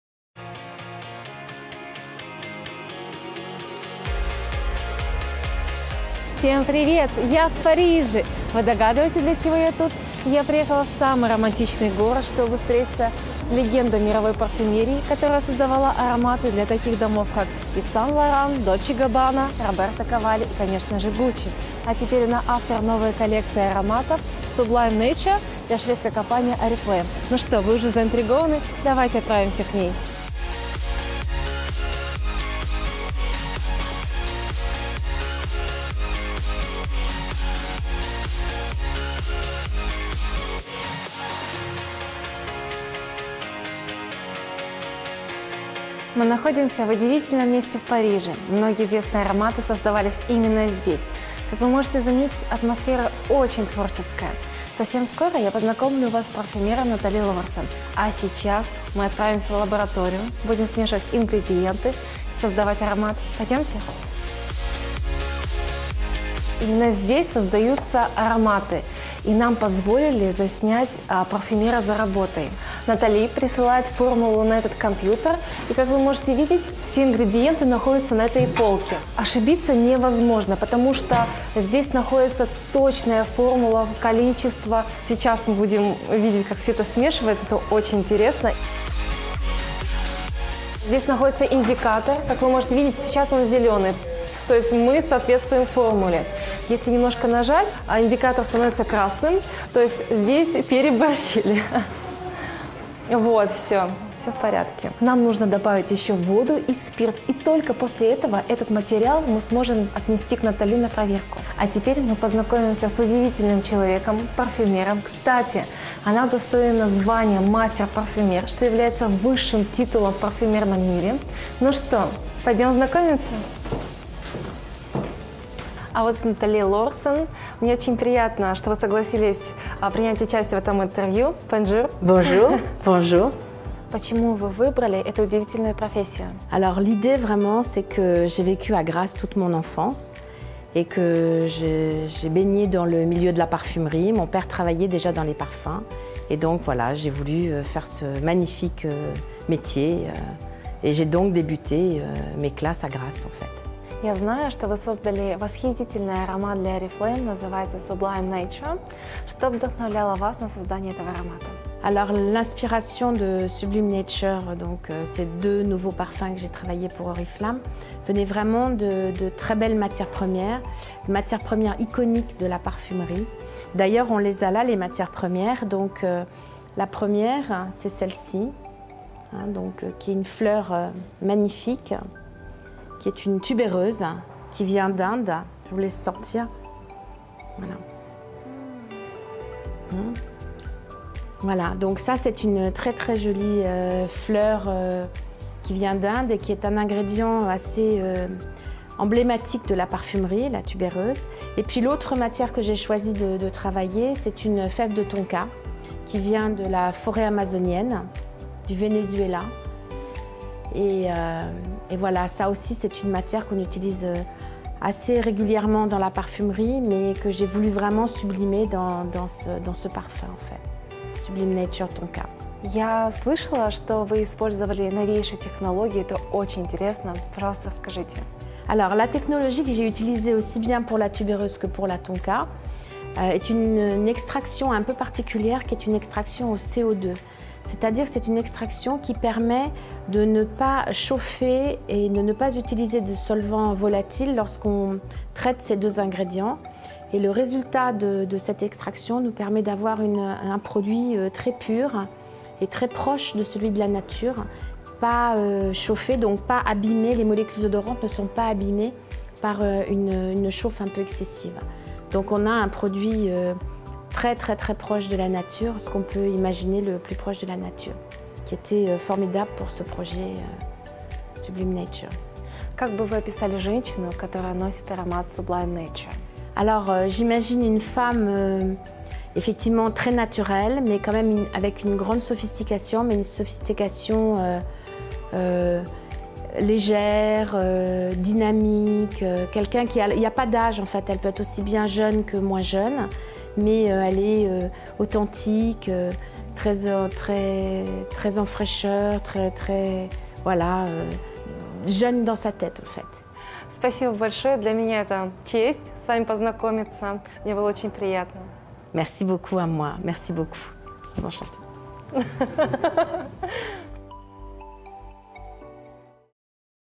Бьюти блогер MissAnnsh в гостях у парфюмера Натали Лорсон в Париже
Эксклюзивное интервью знаменитого парфюмера Натали Лорсон специально для Oriflame! Блогеру MissAnnsh даже удалось побывать в лаборатории, где создаются шедевры парфюмерного искусства. Смотрите скорей!
Категория